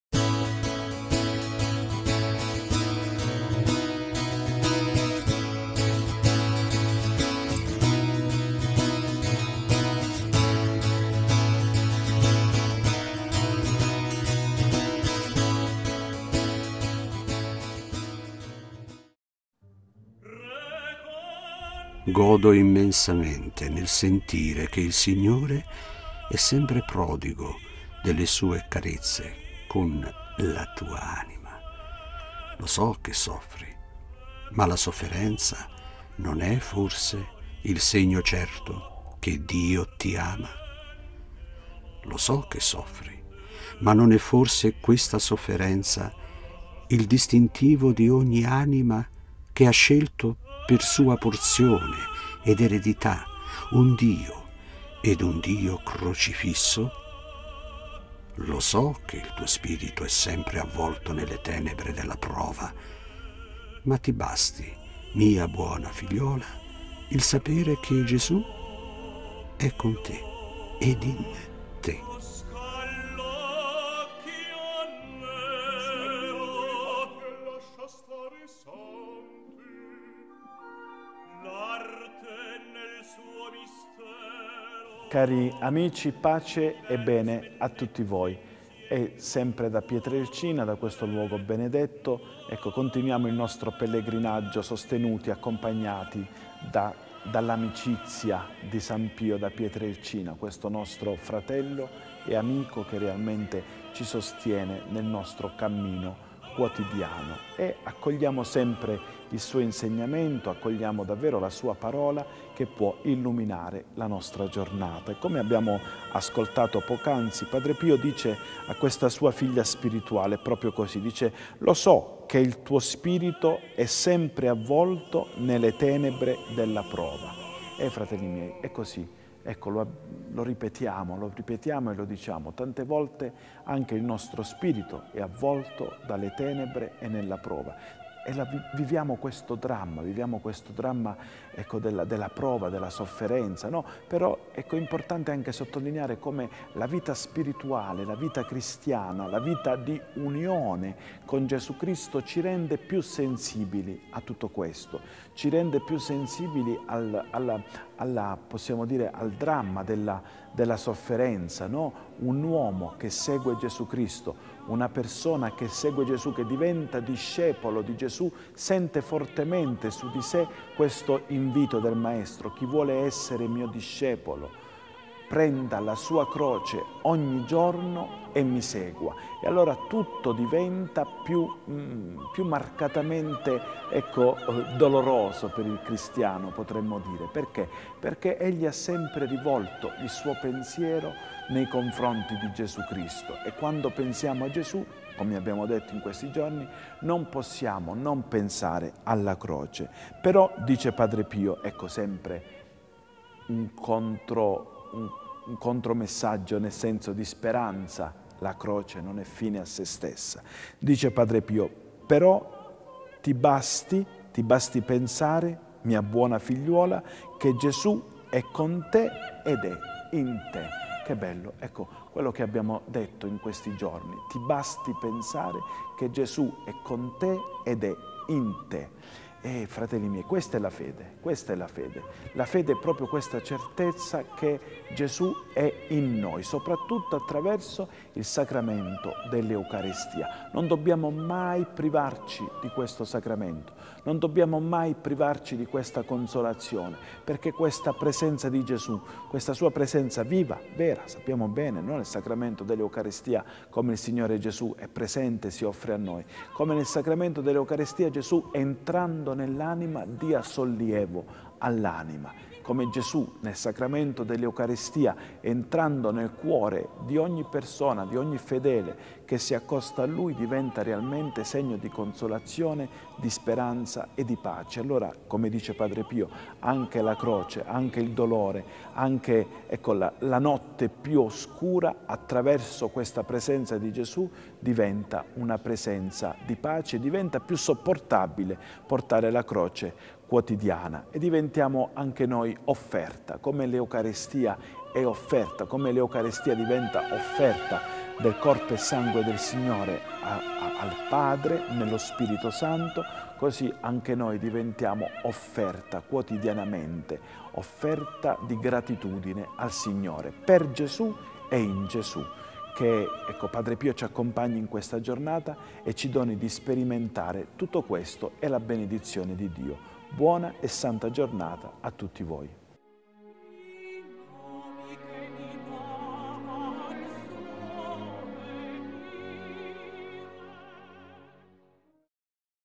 Buona Giornata con Padre Pio 30 Marzo 2020
La Frase di Padre Pio:
"Godo immensamente nel sentire che il Signore è sempre prodigo
delle sue carezze con la tua anima. Lo so che soffri, ma la sofferenza non è forse il segno certo che Dio ti ama? Lo so che soffri, ma non è forse questa sofferenza il distintivo di ogni anima che ha scelto per sua porzione ed eredità un Dio ed un Dio crocifisso? Lo so che il tuo spirito è sempre avvolto nelle tenebre della prova, ma ti basti, mia buona figliuola, il sapere che Gesú è con te ed in te"